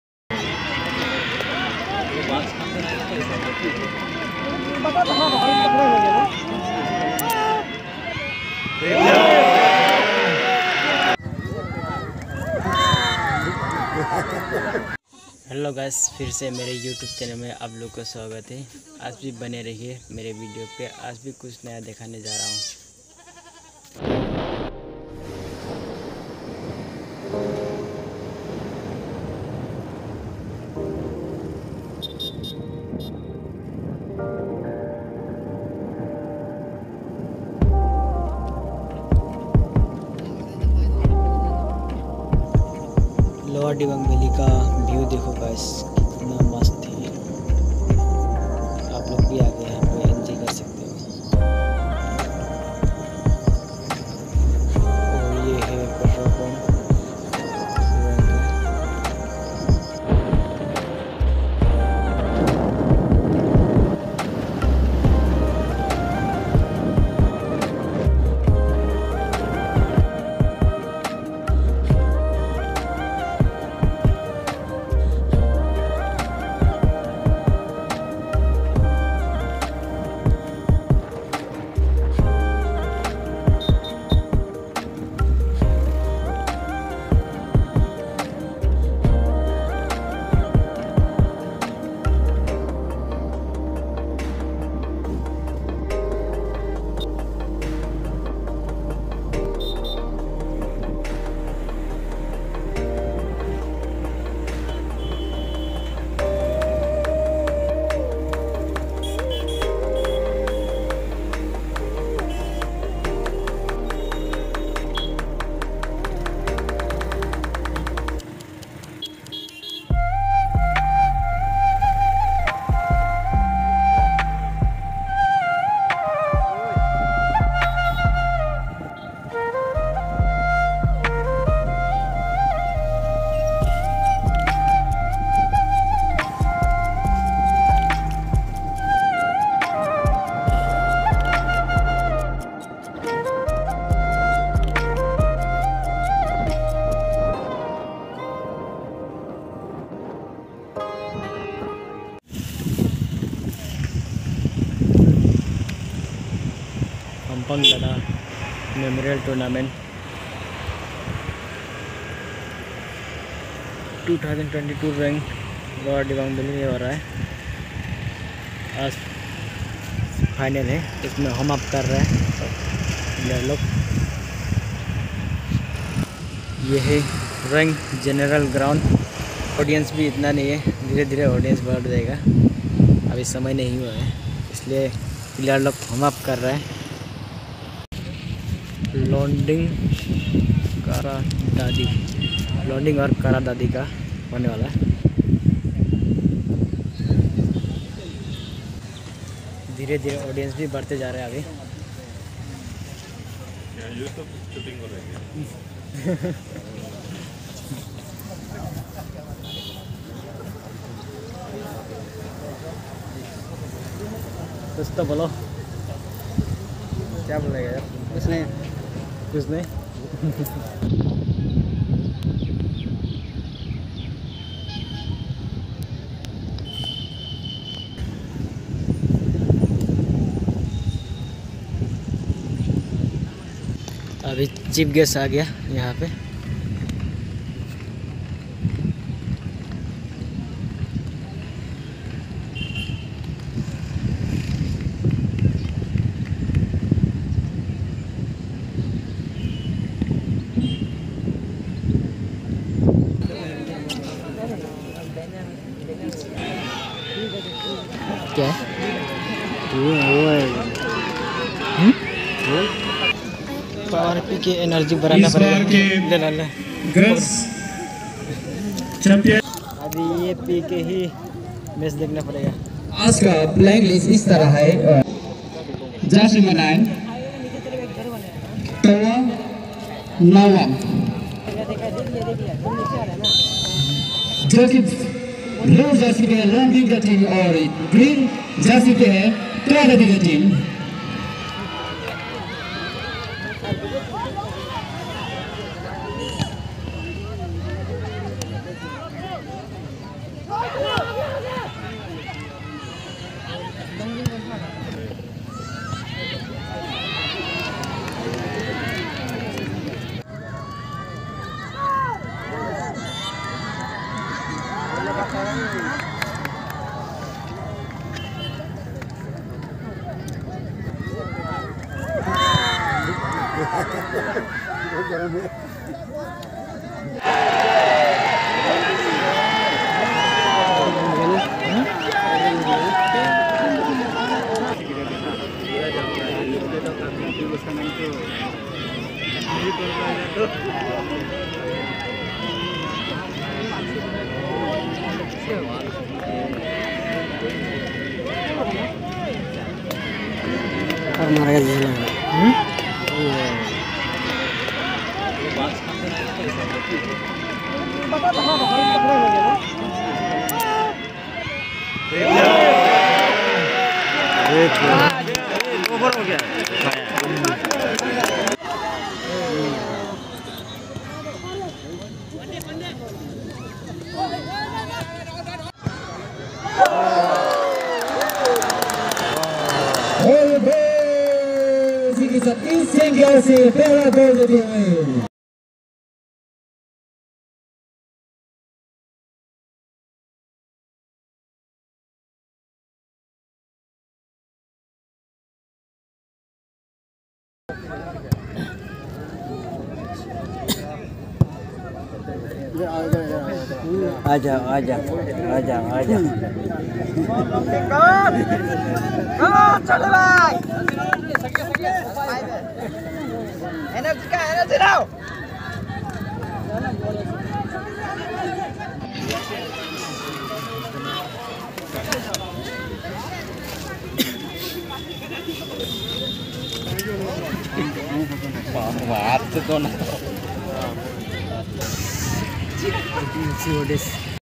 Hangpan Dada memorial trophy
Hangpan Dada memorial trophy 2022 .
Roing general ground football final hangpan Dada memorial trophy